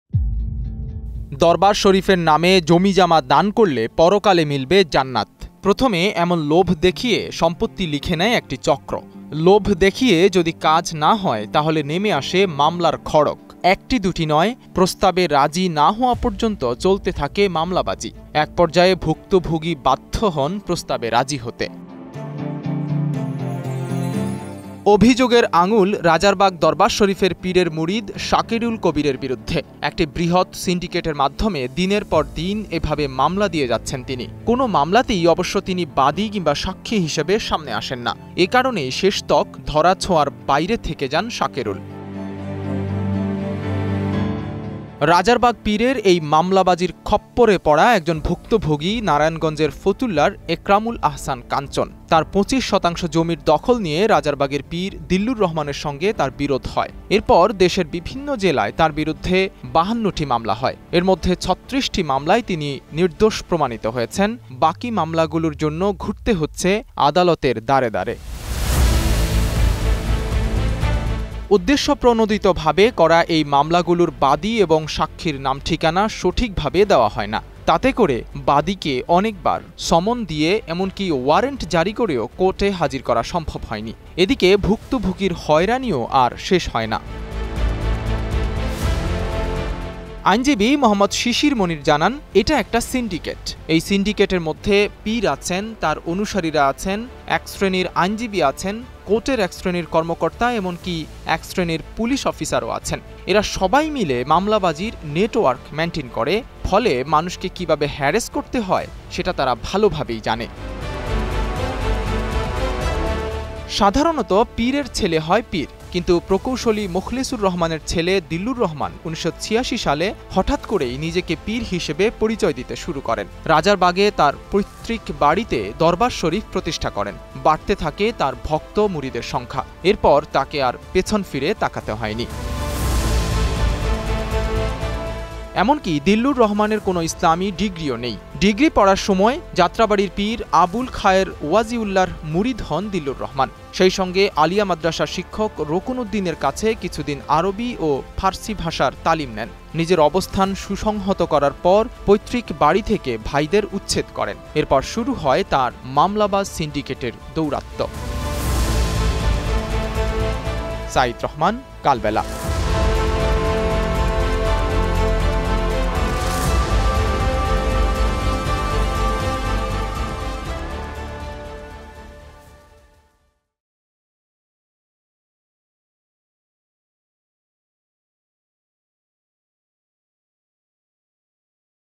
জমির বিনিময়ে জান্নাতের টিকিট | Ticket to Jannat | Rajarbag Darbar Shareef
দরবার শরিফের নামে জমিজামা দান করলে পরকালে মিলবে জান্নাত—প্রথমে এমন লোভ দেখিয়ে সম্পত্তি লিখে নেয় একটি চক্র। লোভ দেখিয়ে যদি কাজ না হয়, তাহলে নেমে আসে মামলার খড়গ। একটি দুটি নয়, প্রস্তাবে রাজি না হওয়া পর্যন্ত চলতে থাকে মামলাবাজি। এক পর্যায়ে ভুক্তভোগী বাধ্য হয় প্রস্তাবে রাজি হতে।